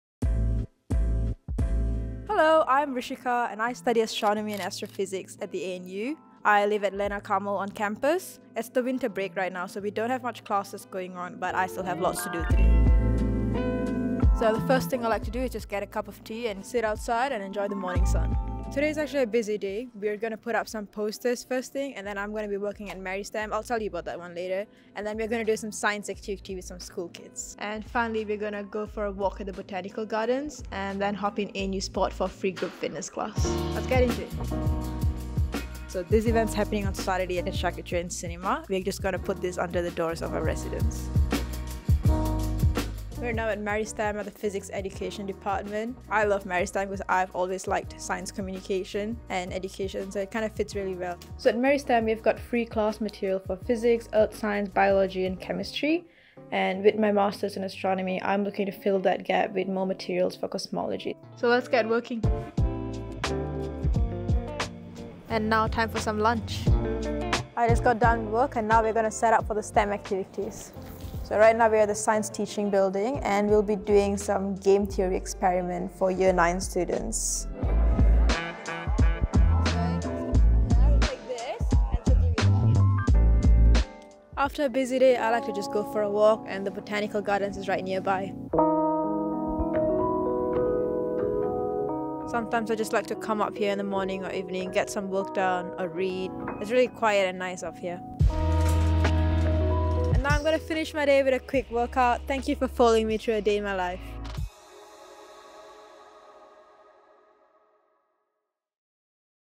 A day in the life of a science student at ANU
Rishika, who is studying astronomy and astrophysics, takes us through her busy day on campus 🔭